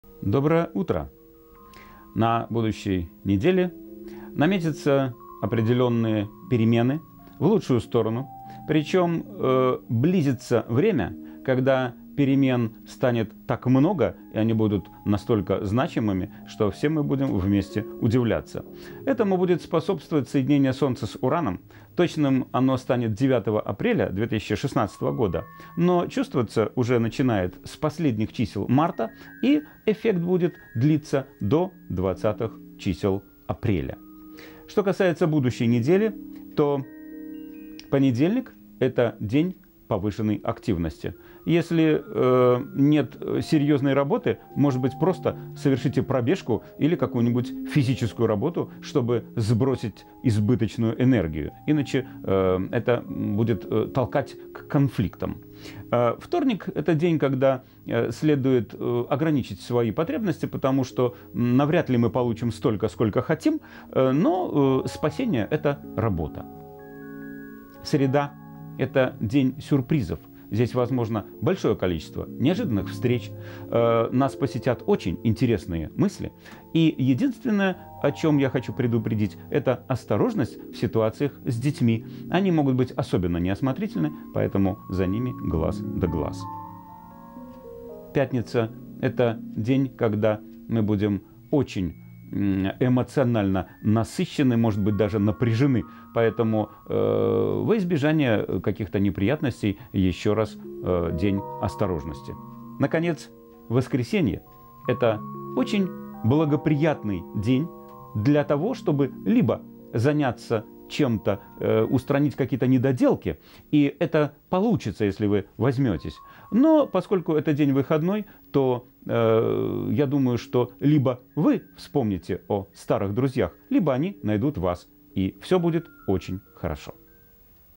Прогноз астролога для деловых людей. 28 марта-3 апреля 2016 г.
Рубрика "Планеты" с Александром Соколовым